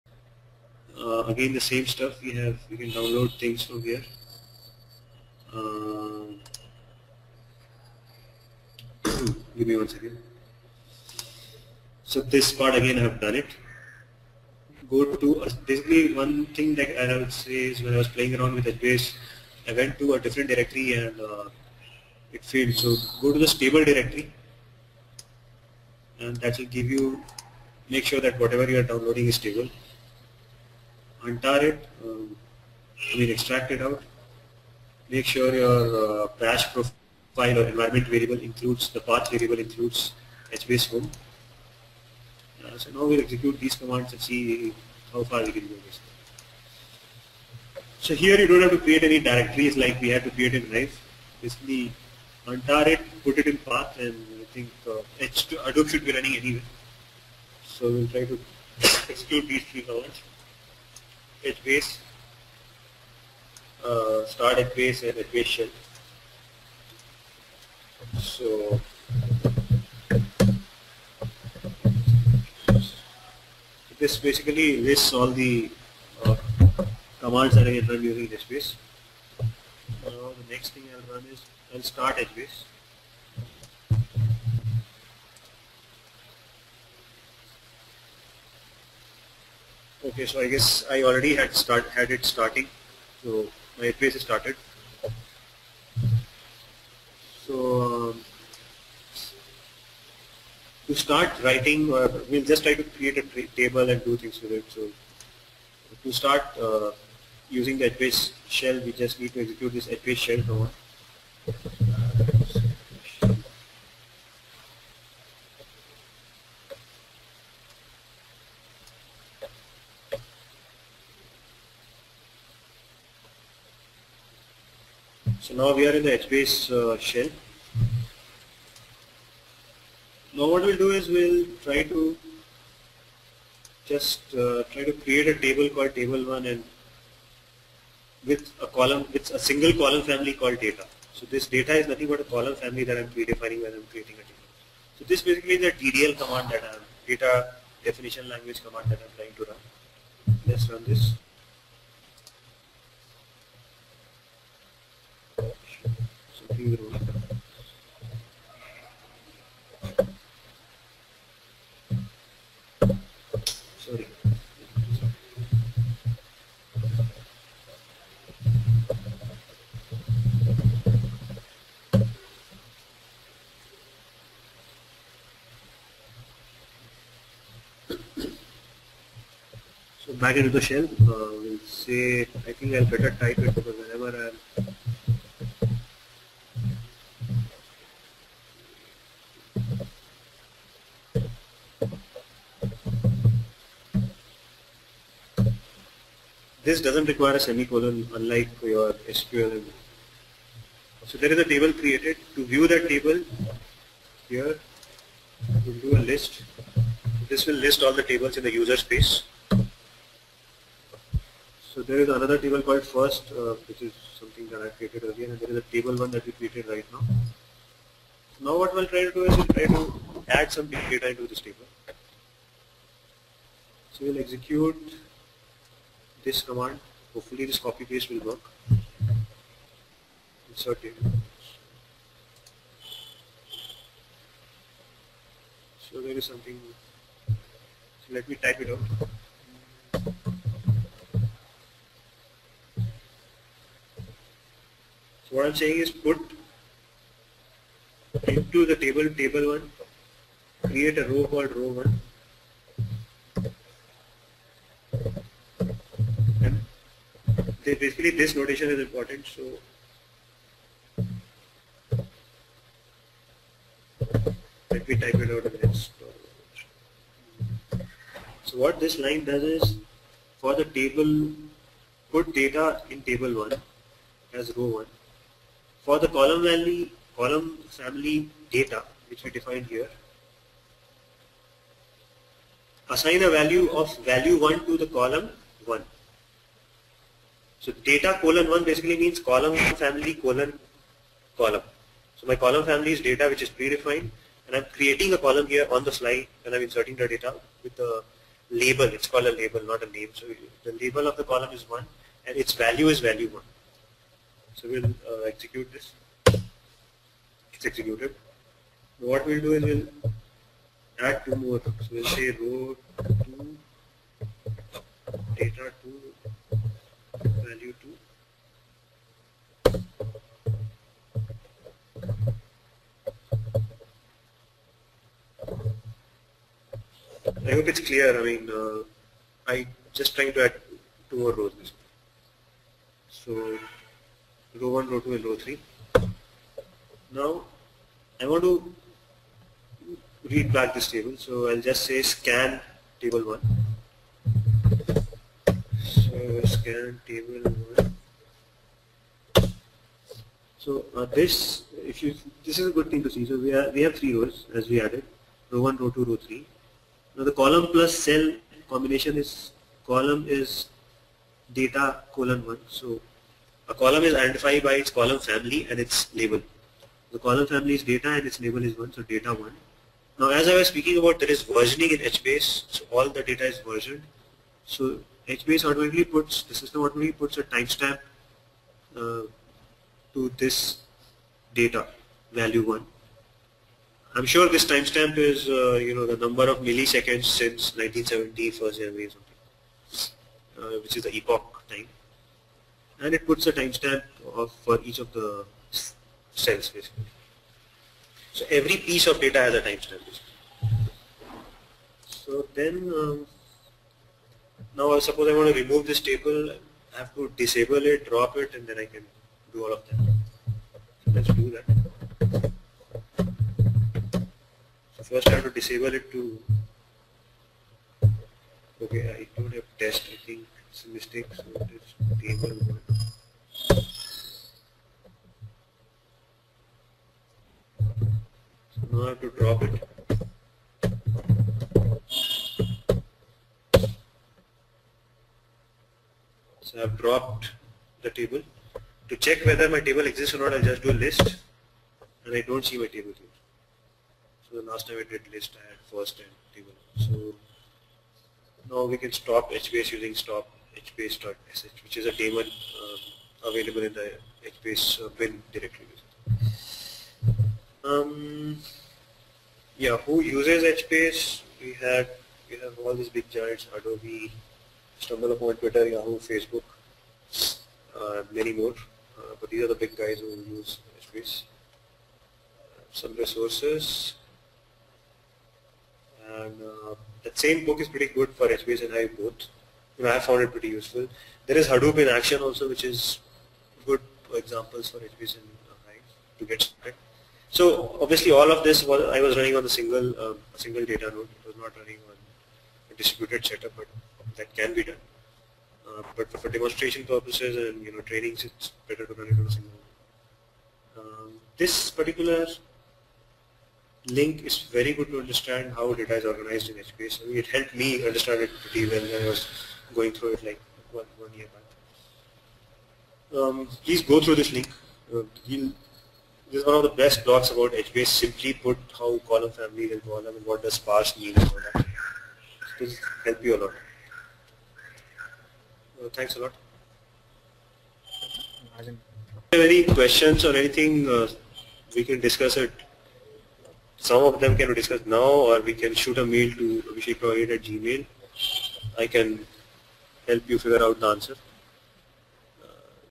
Apache Hadoop Training | HBase Part 2
Apache Hadoop Training - bangalore, Pune, Delhi, Hyderabad

Apache HBase is the Hadoop database, a distributed, scalable, big data store. HBase is an open source, non-relational, distributed database modeled after Google's BigTable and is written in Java